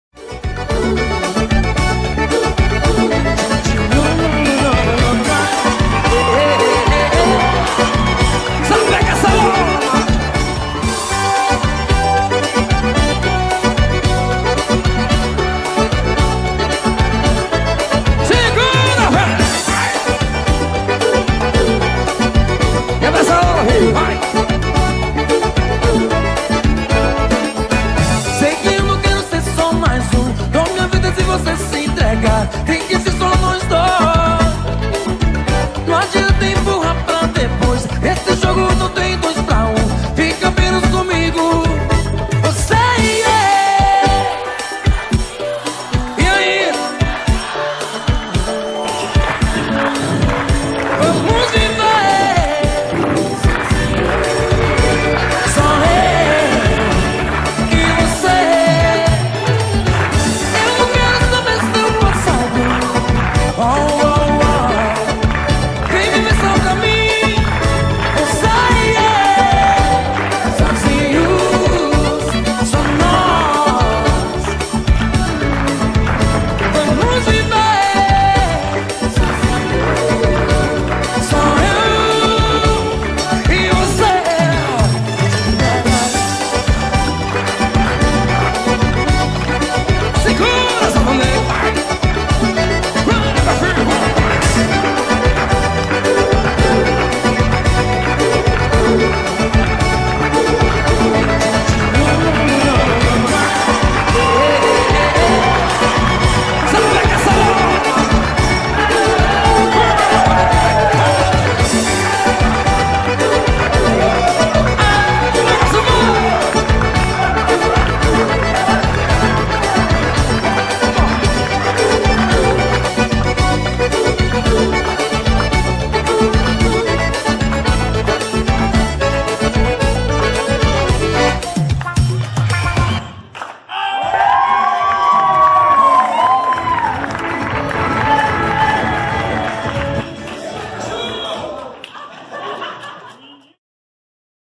WASHINGTON BRASILEIRO - Kuque e Marcela - Forró Eletrônico - Bailando Brasill 2 anos
EVENTO MARAVILHOSO REALIZADO EM GOIÂNIA - GO
REALIZADO PELO GRUPO BAILANDO BRASIL
PROFESSORES KUQUE E MARCELA